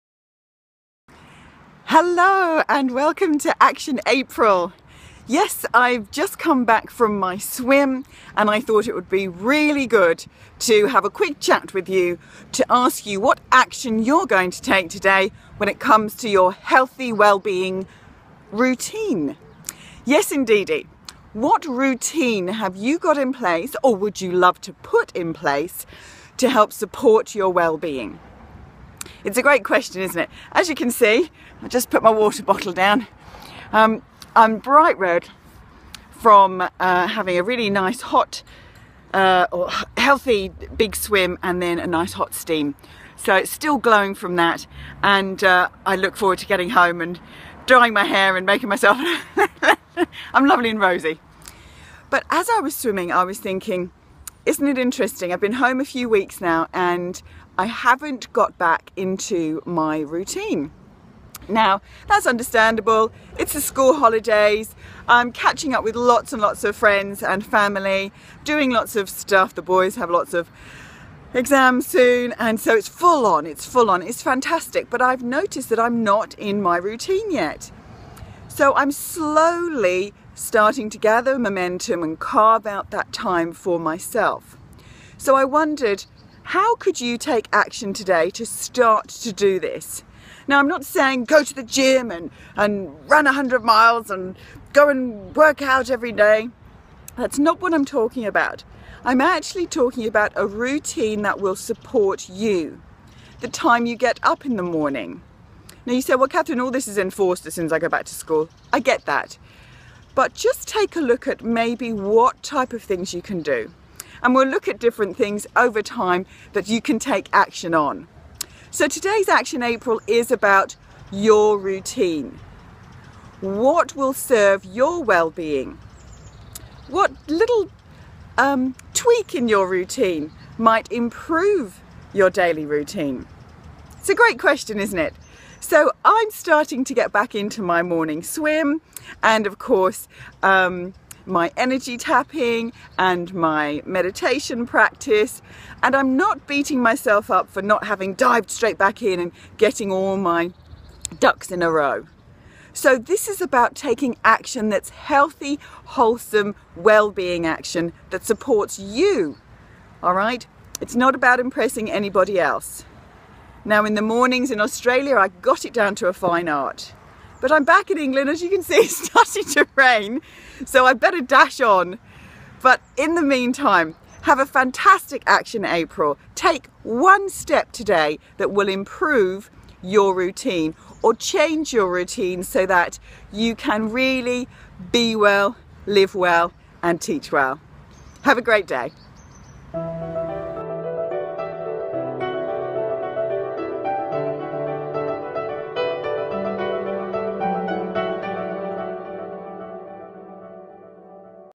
ActionApril! Day 4
Do you clean your teeth every day? Yes of course, it's part of your routine right? Join Kathryn Lovewell from The Teacher Sanctuary exploring what action you can take to develop or improve your Wellbeing routine.